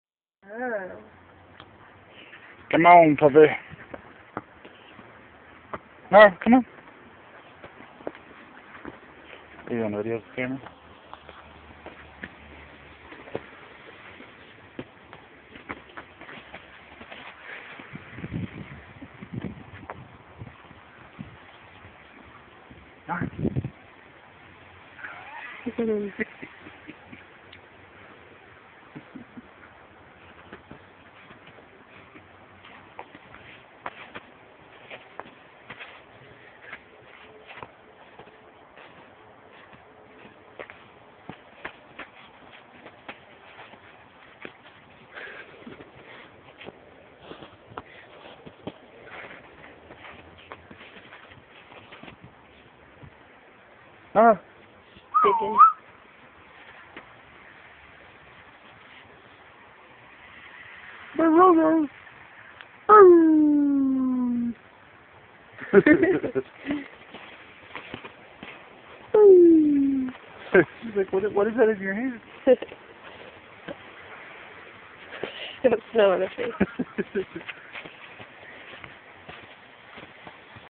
Nara's snow times
Our little blue heeler enjoying the snow